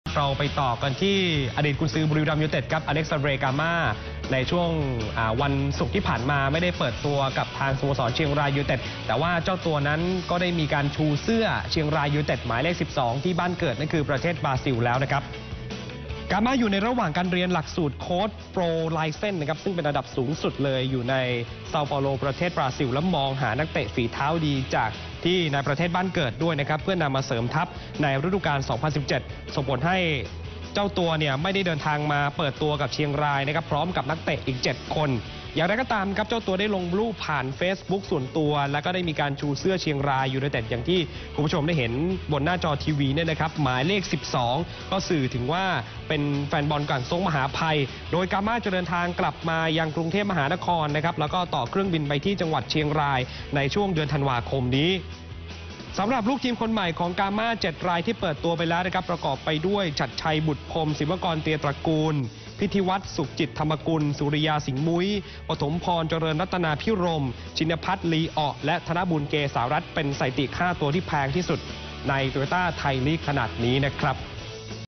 "กาม่า" เปิดตัวถือเสื้อเชียงรายจากบราซิล
พิกัดกีฬา ช่วงค่ำ : ความเคลื่อนไหวไทยลีก "กาม่า" เปิดตัวถือเสื้อเชียงรายจากบราซิล "NOW26" 5-11-59